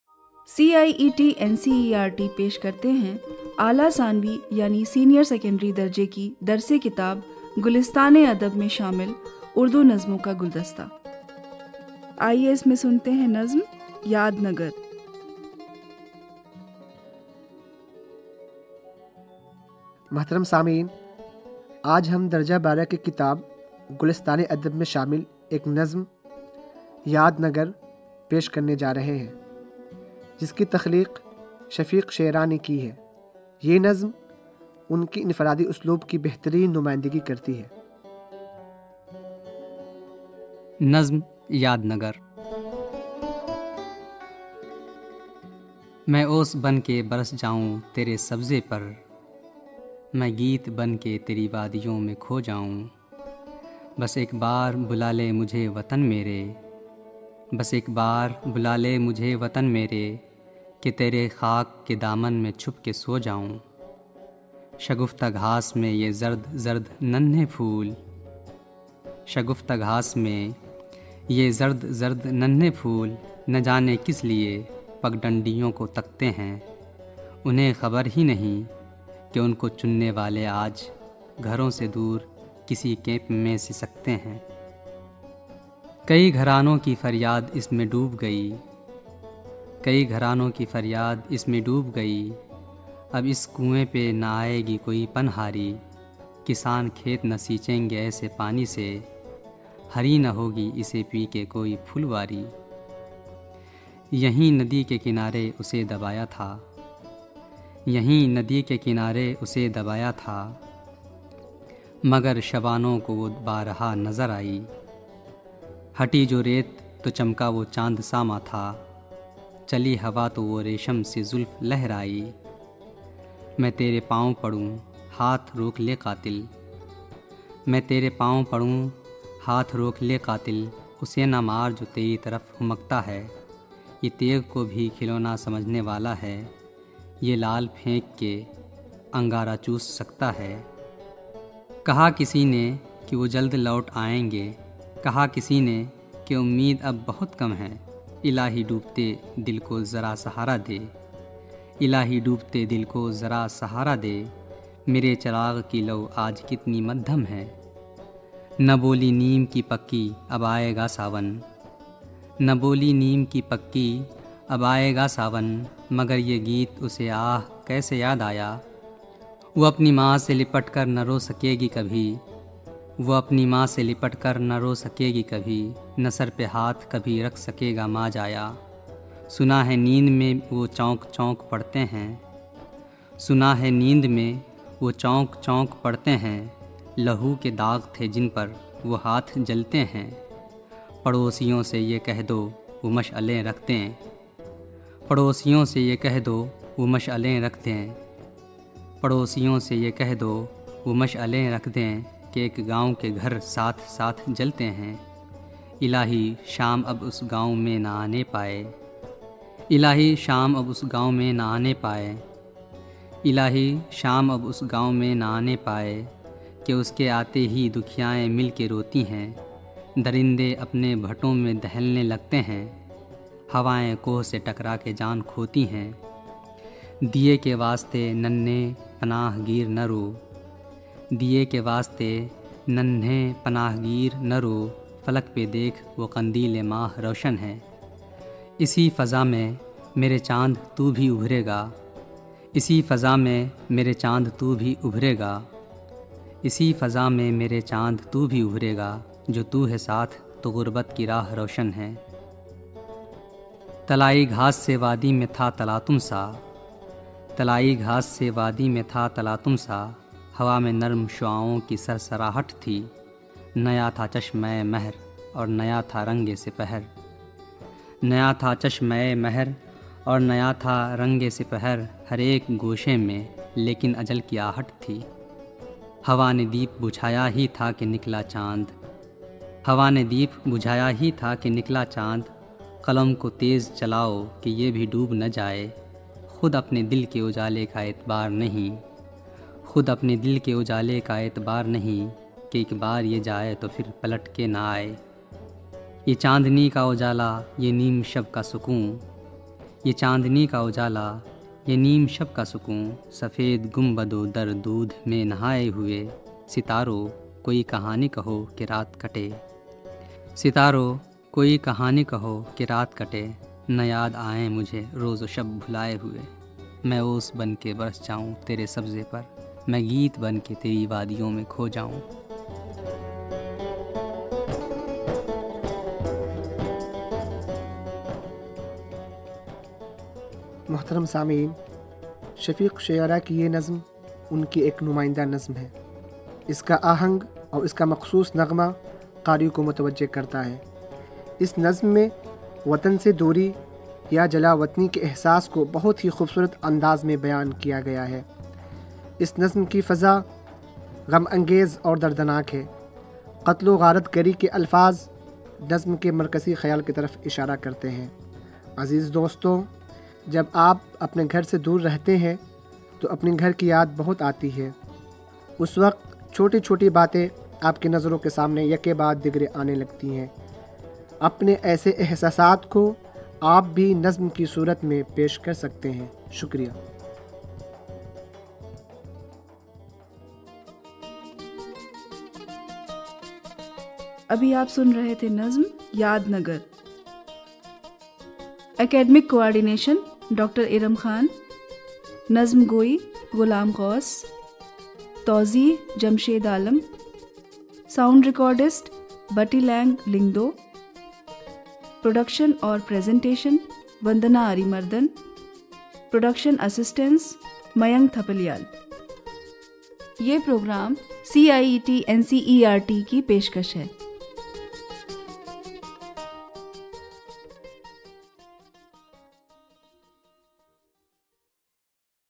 यादनगर / Yaadnagar
ये कार्यक्रम एक नज़्म है जो की गुलिस्तां-ए-अदब, ncert की पाठ्यपुस्तक से